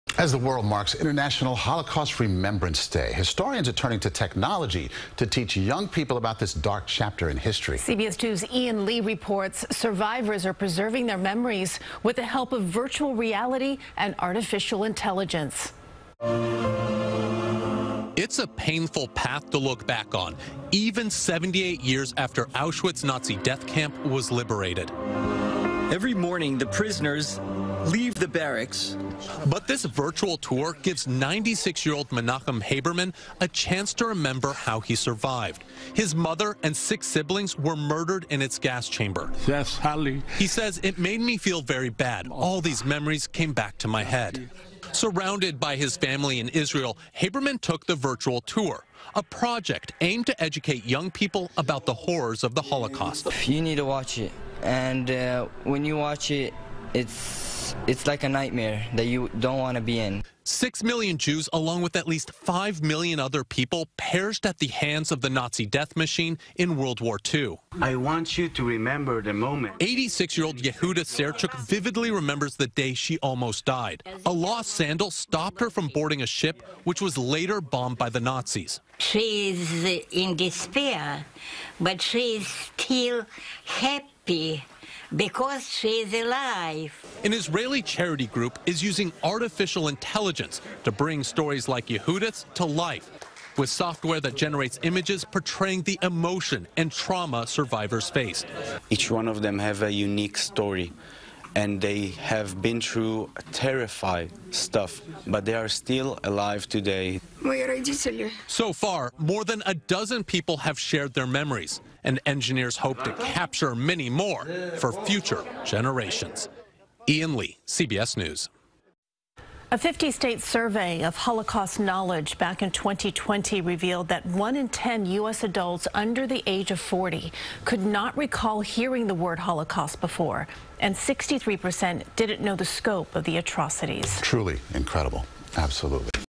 Historians using VR, AI to educate young people about the Holocaust
As the world marks International Holocaust Remembrance Day, historians are turning to technology to teach young people about the dark chapter in history. Ian Lee reports for CBS2.